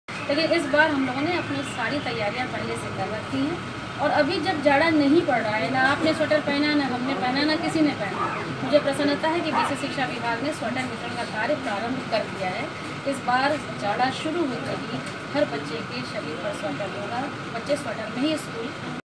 इस बार नौनिहालों को स्वेटर के लिये नहीं करना पड़ेगा इंतजार : अनुपमा जायसवाल
बाराबंकी: पिछले वर्ष ठिठुरते हुये स्कूल जाने वाले परिषदीय स्कूलों के बच्चों को इस जाड़े में स्वेटर के लिये इंतजार नहीं करना पड़ेगा. विभाग ने समय पर ही स्वेटर बांटने की तैयारी कर ली है. यह कहना है बेसिक शिक्षा मंत्री अनुपमा जायसवाल का.